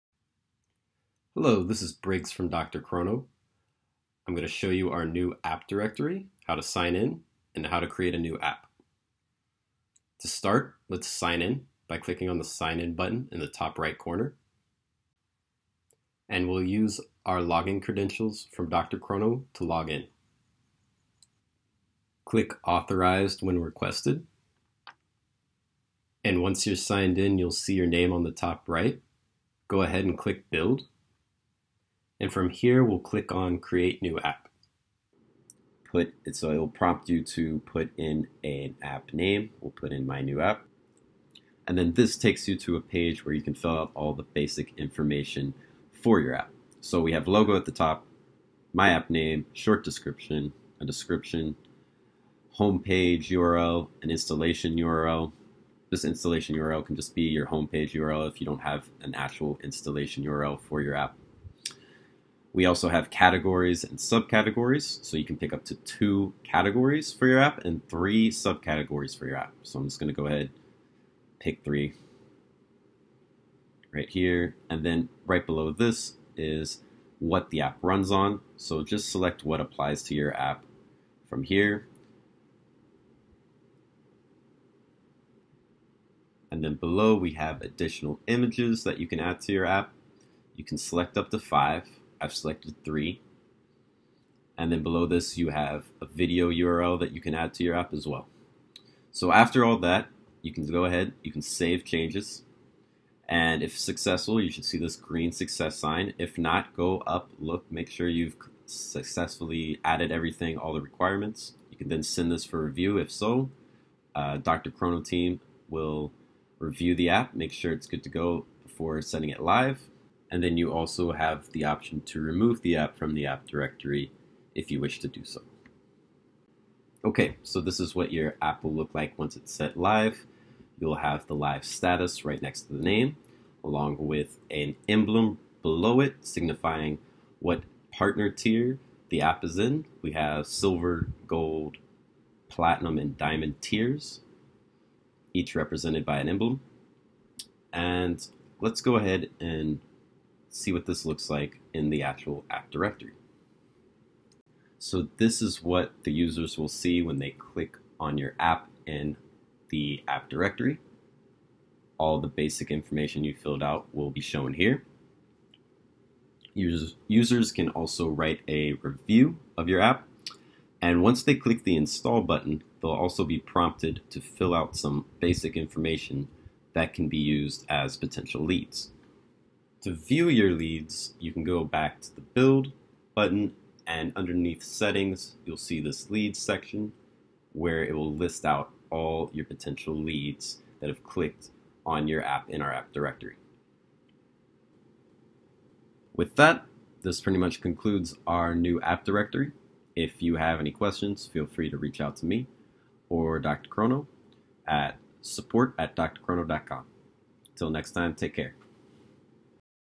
For Developers: Healthcare Partner Platform Marketplace // DrChrono - Launch your app!
How to create an app with DrChrono's new app marketplace.

Build Something Amazing. Free Healthcare API and SDK built by developers, for developers. Leverage health data, an EHR, practice management and medical billing platform.